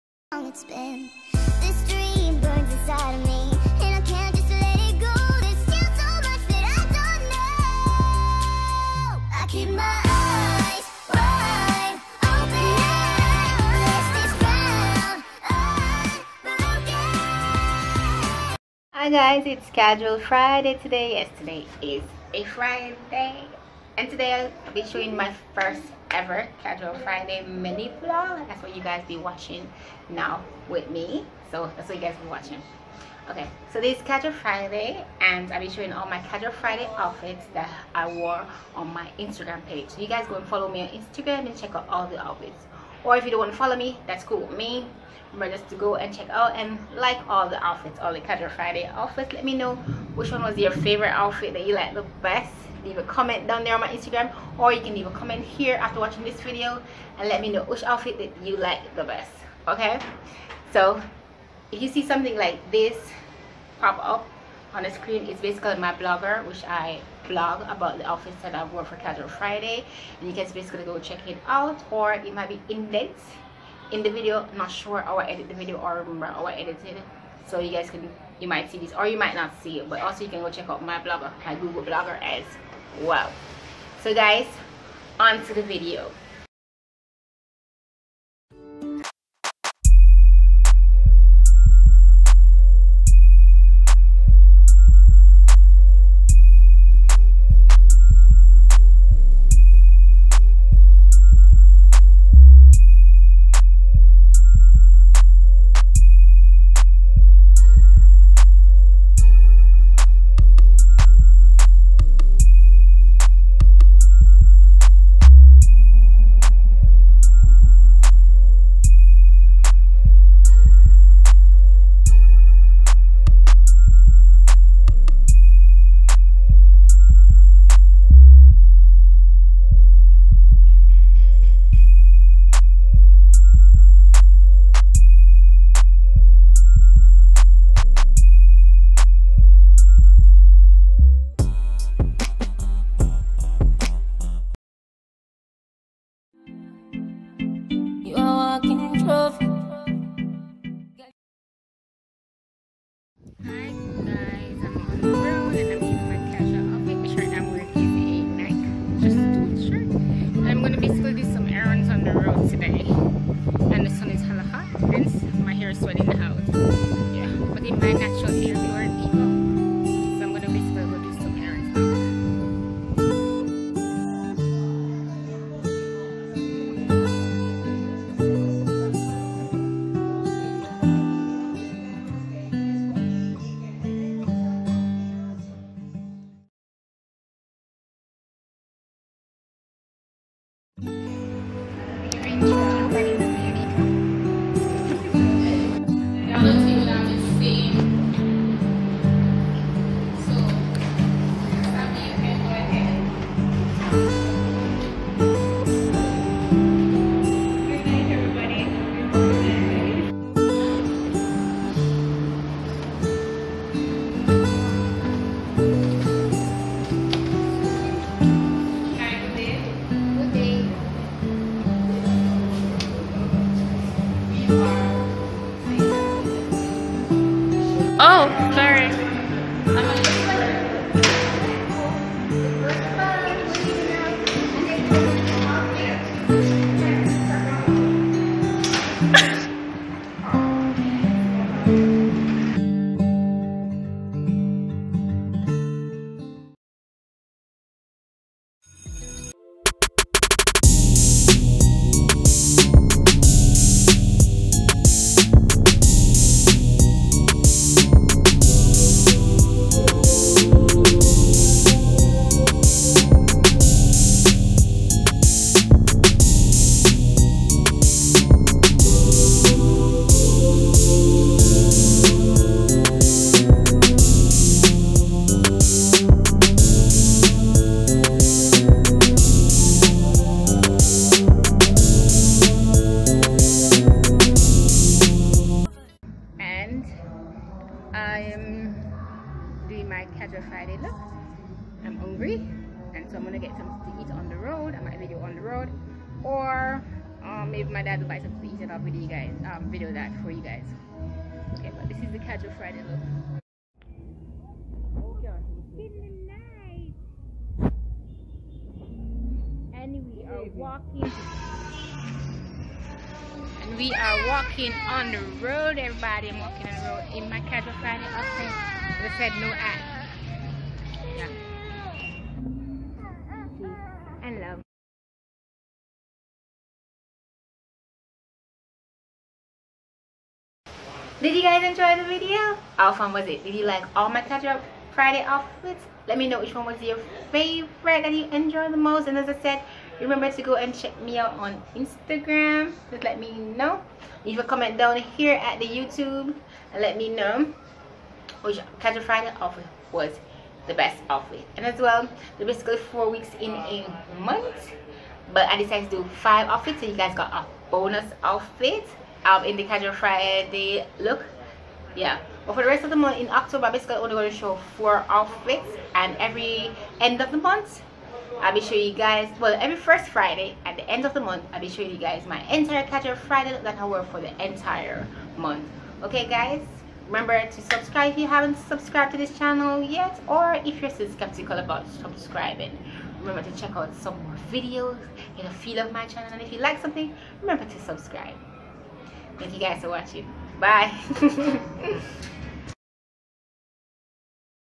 Casual Friday (The Simple Life Lookbook)
Its Casual Friday everyone
Here are some of my Simple Life casual looks, that I wore every Friday of the month of September.